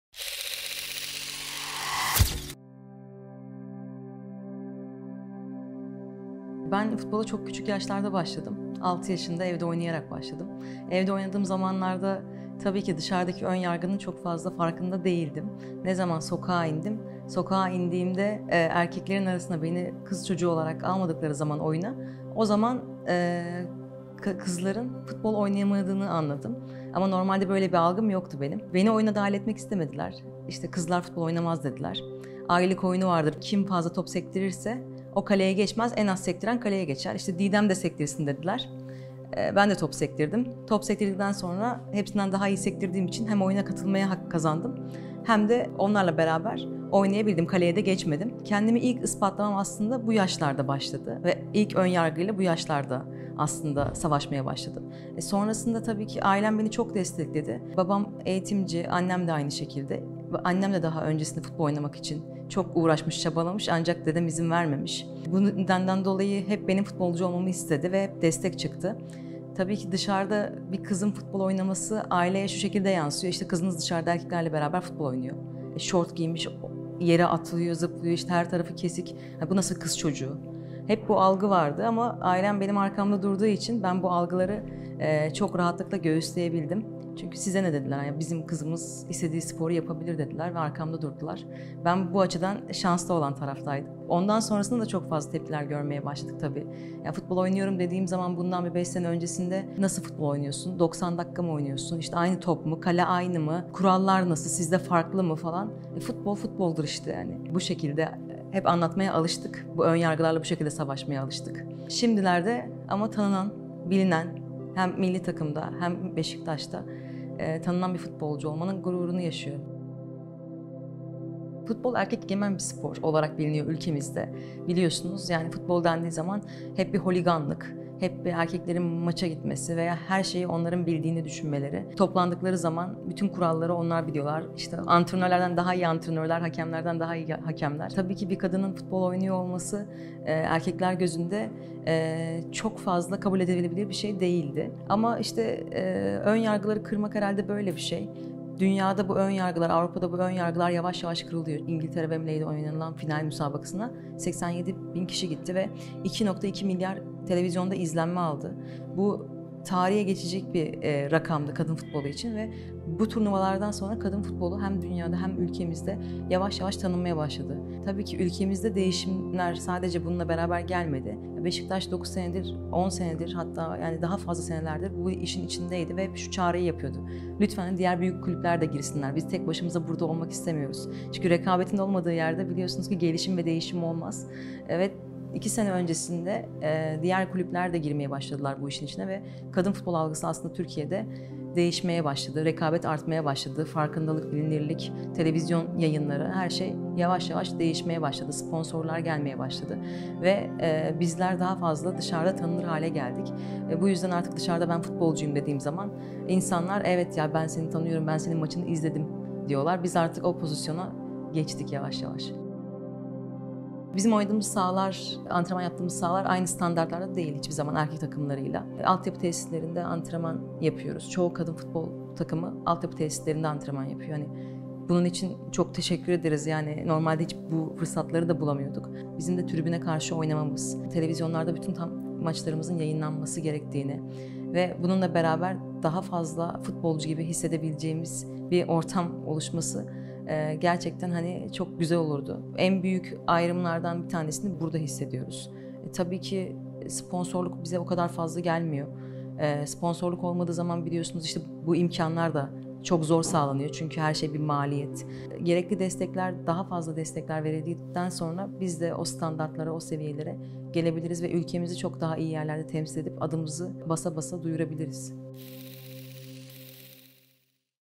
8 Mart 8 Kadın | Didem Karagenç: Futbola dair tüm çirkin görüntüler kadın futbolcularla değişebilir
Topun peşinde koşturmaya, henüz 6 yaşındayken, mahalleden erkek çocuklarla sokakta başladı. 11 yaşındayken lisansını aldı. 15’inde U_15 Milli Takımı formasını giydi ve bir daha da çıkarmadı. 100’ler kulübüne girdi, 118 defa milli takım formasını giyen nadir oyunculardan oldu. Bir dönem Türkiye’de lig maçı oynayamadı çünkü kadın futbol liglerinin yeniden kurulmasını beklemek zorunda kaldı. İlk altı sezonunda, toplam 70 maçta forma giydi ve 32 gol attı. Türkiye A Milli Kadın Futbol takım kaptanı Didem Karagenç, ülkemizin en başarılı kadın sporcularının başında geliyor. 2015’ten beri Beşiktaş Kadın Futbol Takımı'nda yer alan Karagenç, kadınların futbolu nasıl değiştirebileceğini anlatıyor.

Röportaj: Ali Tufan Koç
Video: Studio Faal
Işık Asistanı: Arda Anıl Kara
Proje Koordinatörü: Gülce Güler
Dijital içerik Direktörü: Sercan Sarıkaya